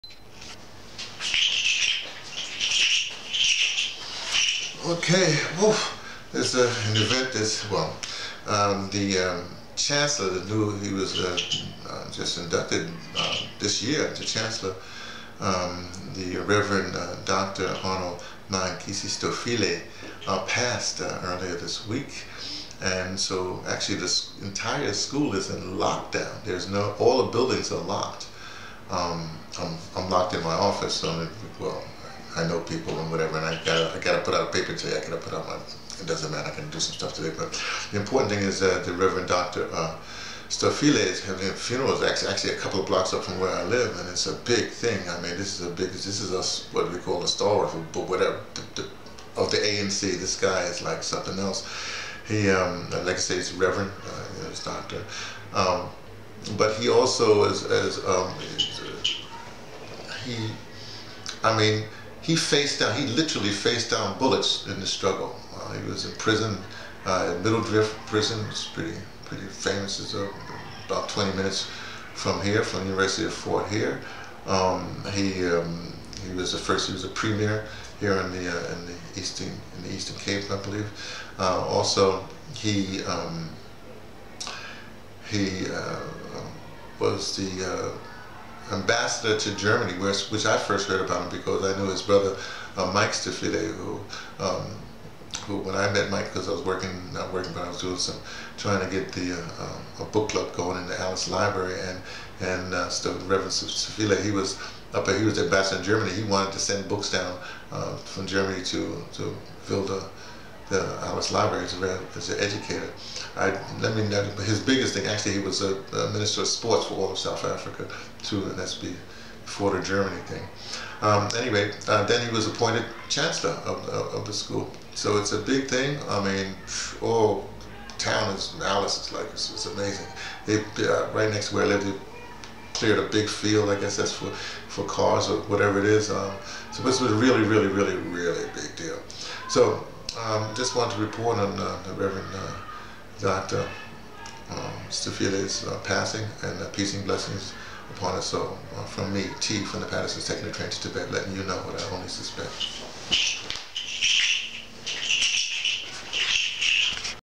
Funeral Lockdown
Noting the passing of Southern African Struggle stalworth: Reverend Dr Arnold Makhenkesi Stofile.
Described as a Humble man, Astute Politician, respected Rugby sports man and Academic.
Peace & Blessings on his eternal Soul.